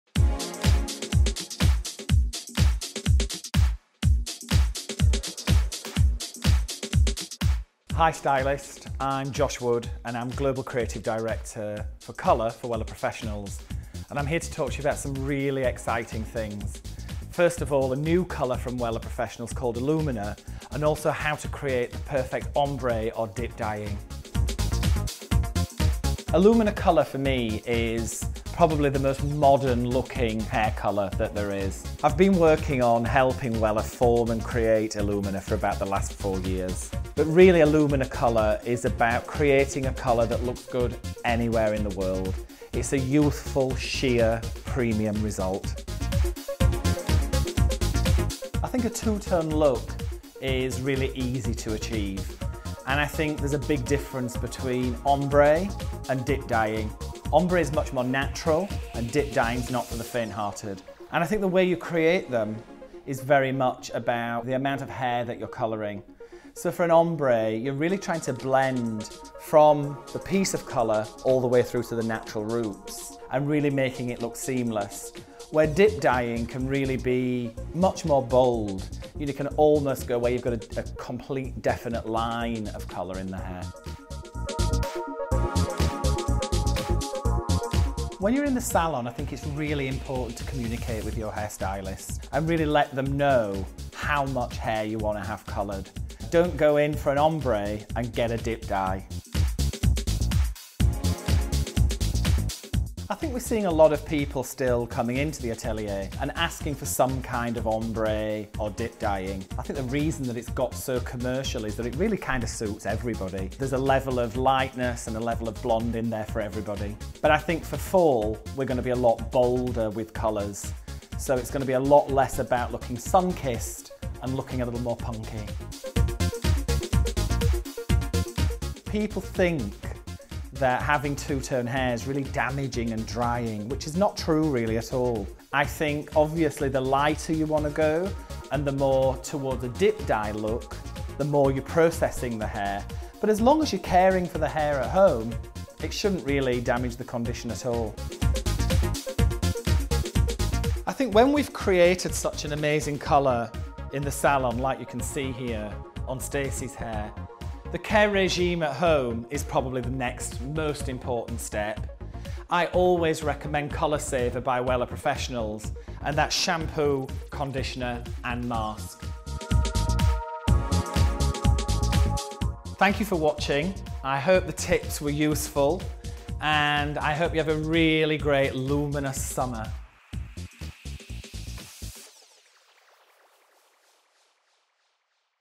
How To Dip Dye Your Hair With Josh Wood
Wella's hair colour expert Josh Wood answers common questions about going for dip-dyed hair in this Q+A hair tutorial video.